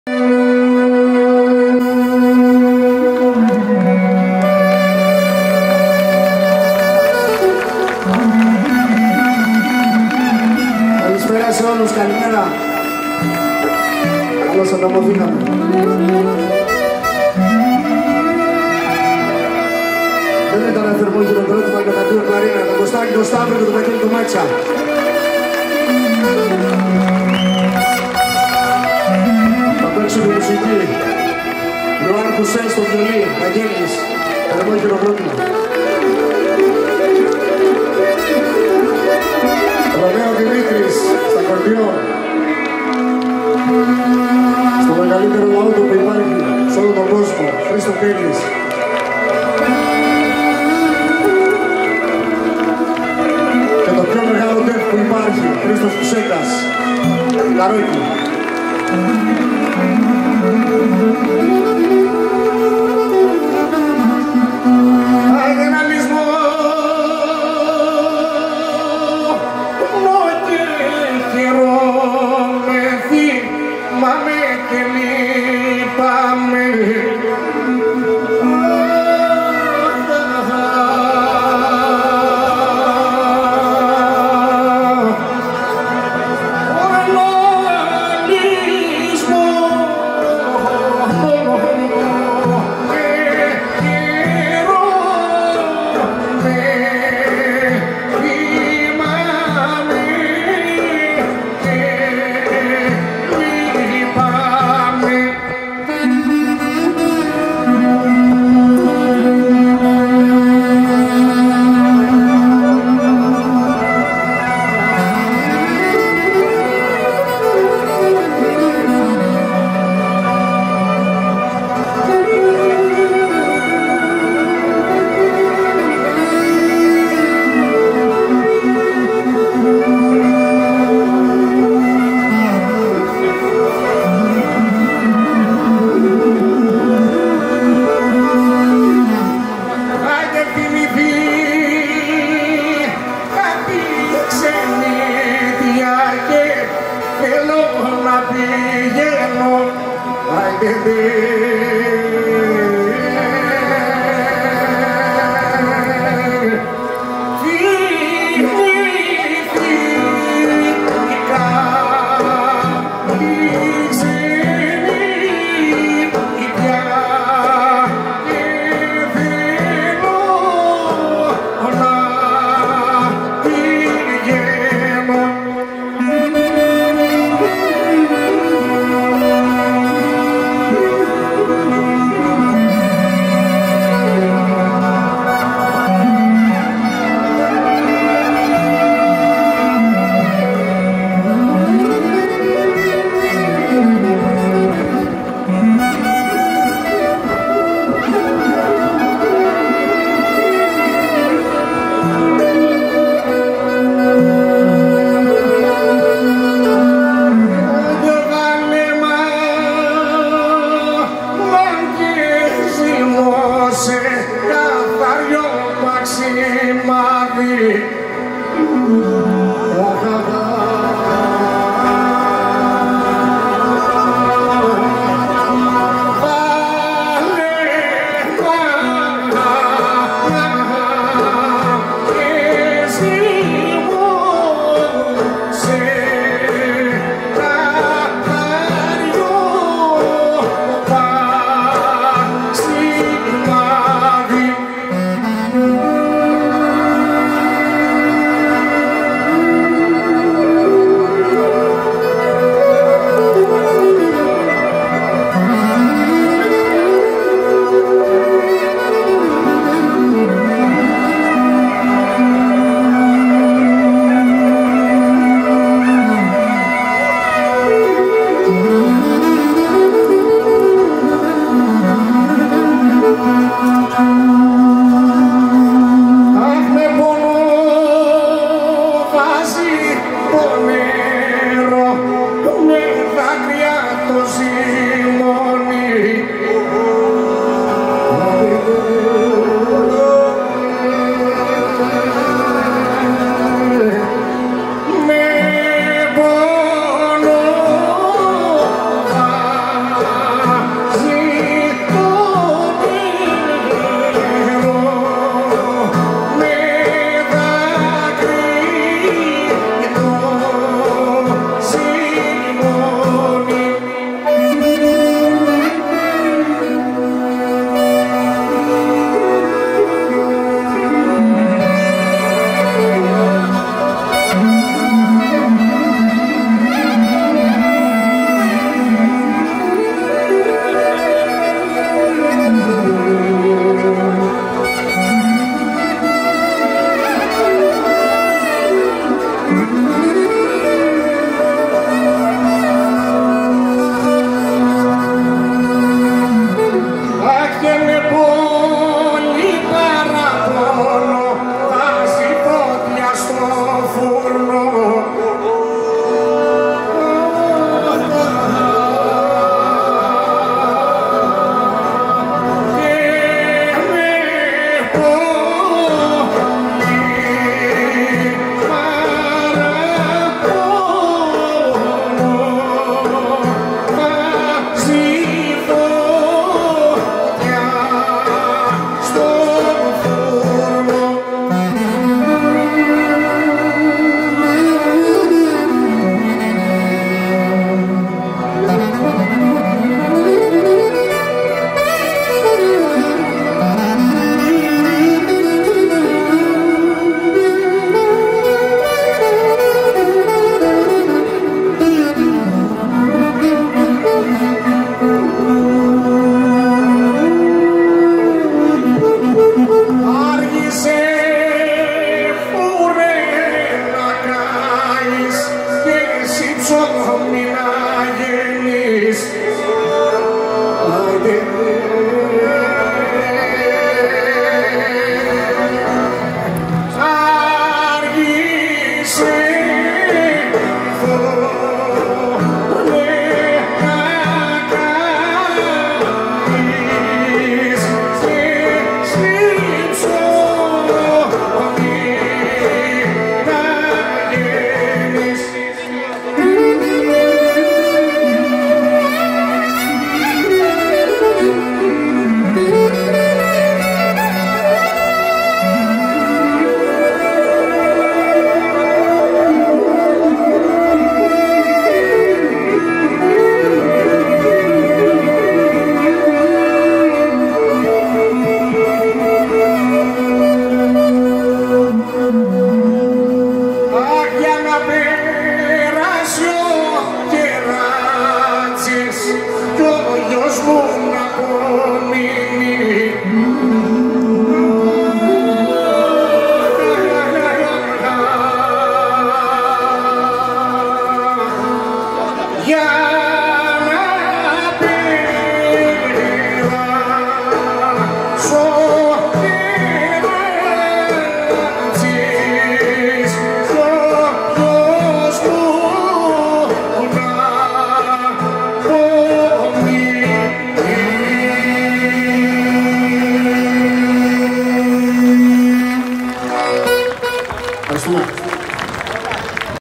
Δ.Ε.Ε.Ε.Μ. - ΟΜΟΝΟΙΑ 2ο Παν - Βορειοηπειρώτικο Αντάμωμα 29-5-2022 βίντεο 1
Μουσικοί :
Τραγούδι: Χρήστος Αγγελής
Κλαρίνο: Κώστας Σταύρου - Βαγγέλης Μάτσιας
Βιολί: Luan Huse
Ακορντεόν: Romeo Dhimitri
Λαούτο: Keli Bame
Ντέφι: Χρήστος Φουσέκας